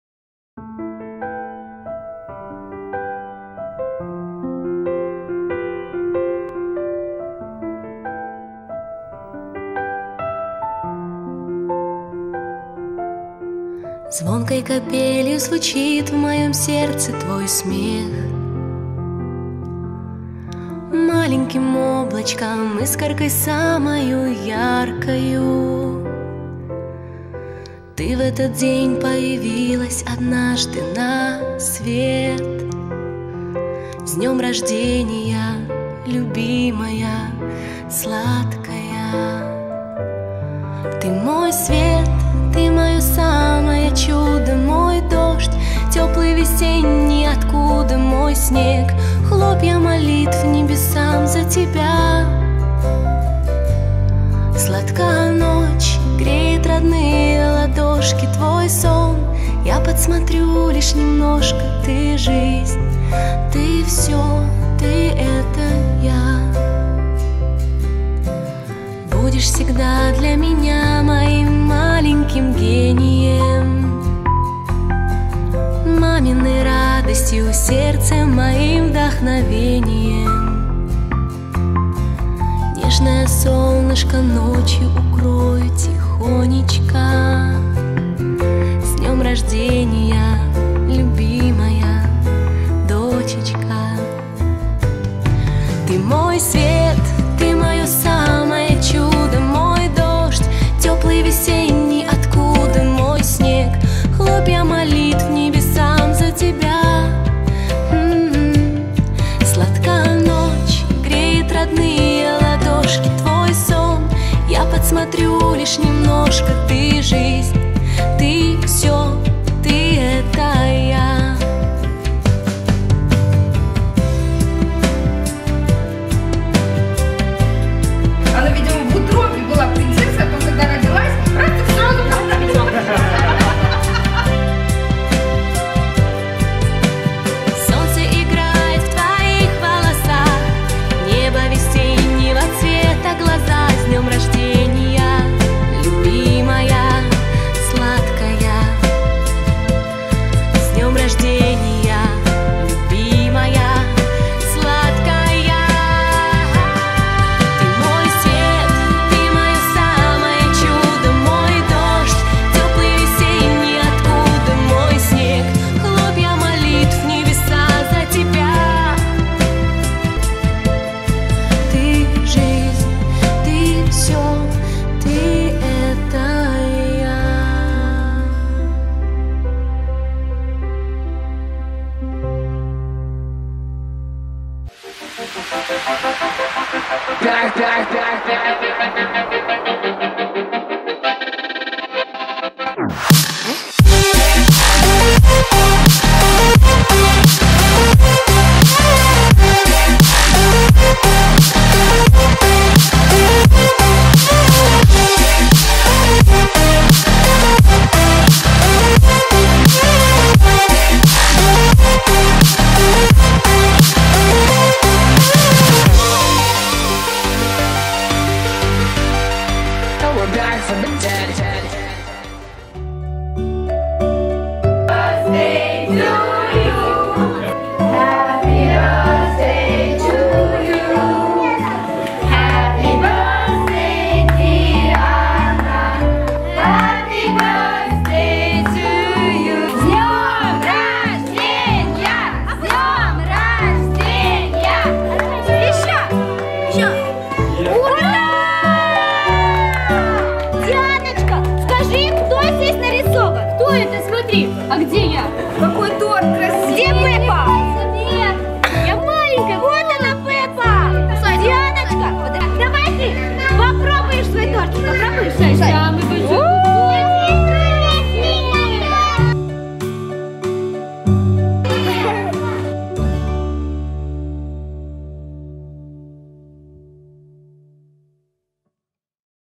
Birthday of Princess Diana! Дианочке 3 года!
17 января 2014 года на свет появилась наша долгожданная принцесса! Наша Леди Ди! Отмечать день рождения дочи в нашем Монако уже стало традицией. В этом году Диана захотела отметить свой день в окружении Принцесс Эльзы, Софии, Анны, Золушки, Феи и своих друзей.